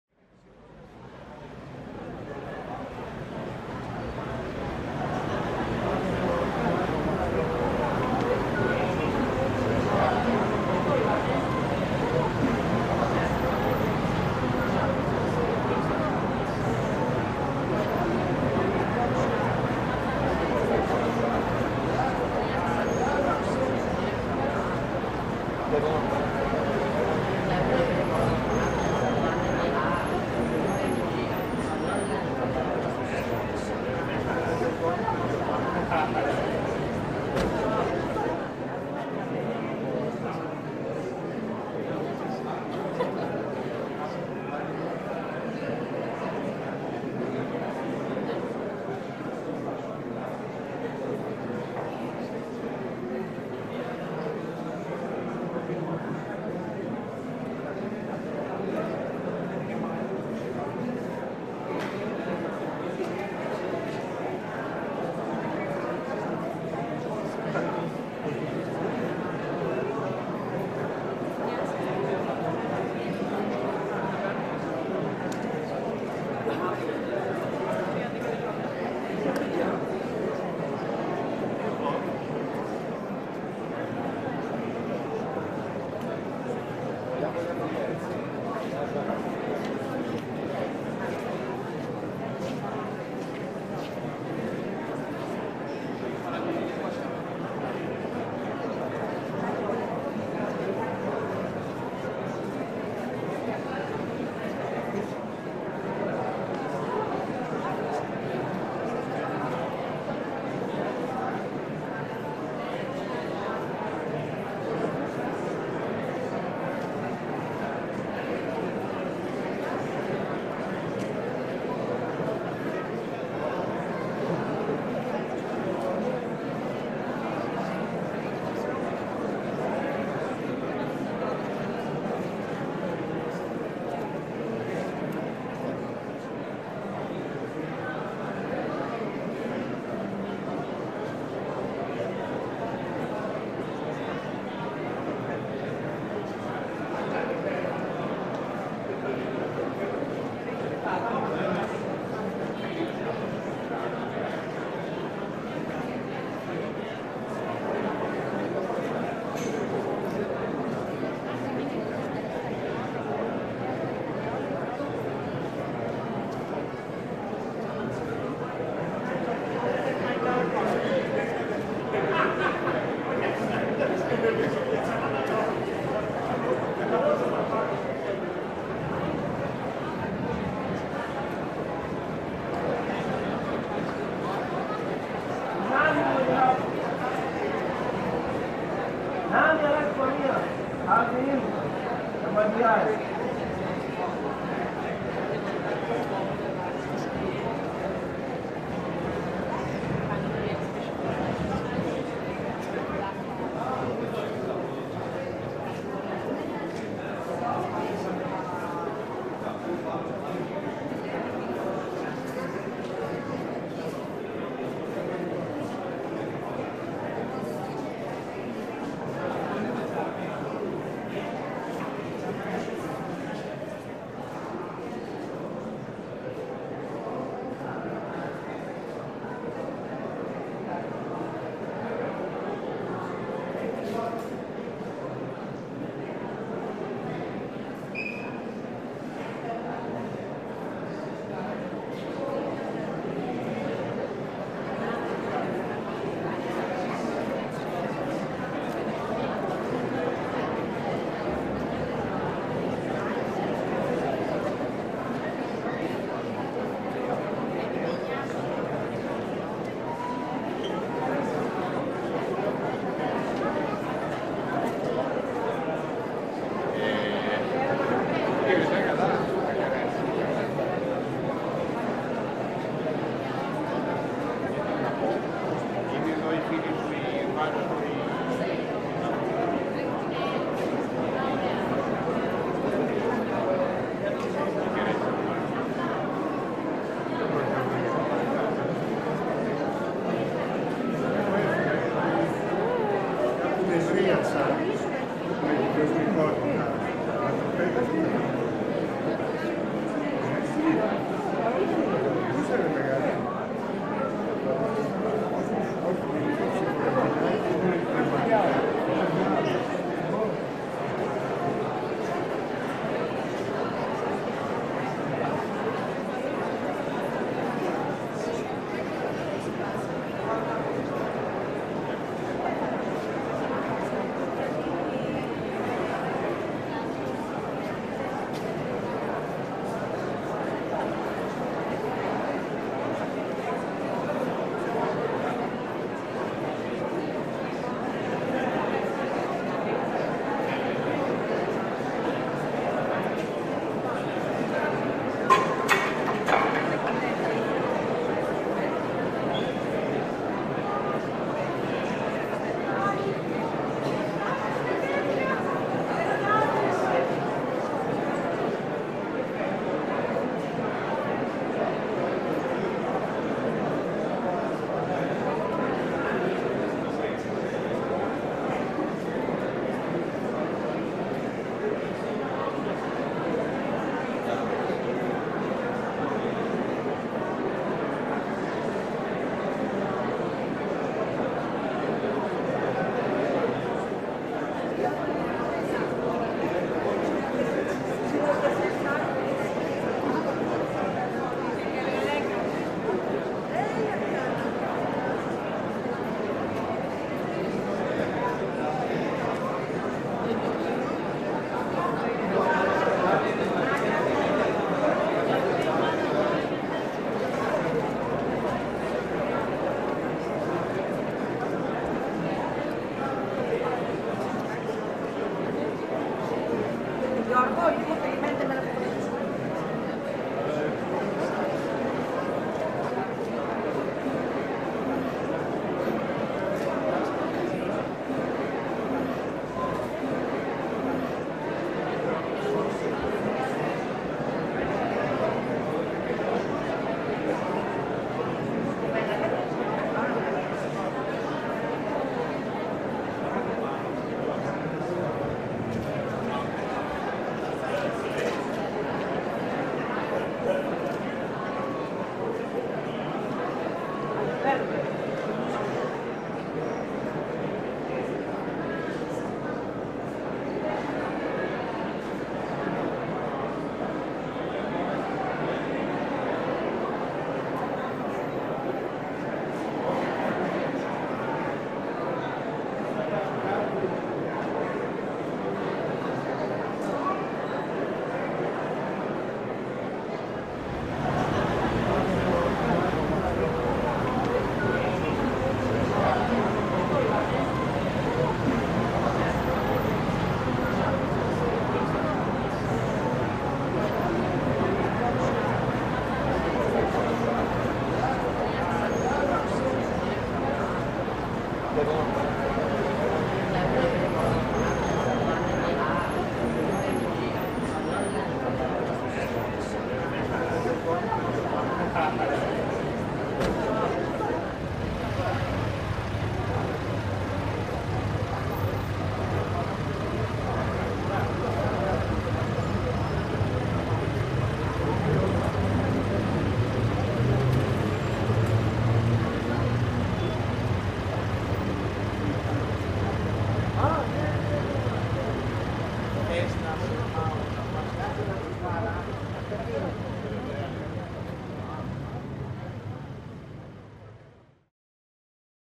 DOCTVGR 360° VIDEO: EMST
Οι Αθηναίοι καλωσορίζουν το δικό τους Εθνικό Μουσείο Σύγχρονης Τέχνης. Μπορείτε να περιηγηθείτε σε 360 μοίρες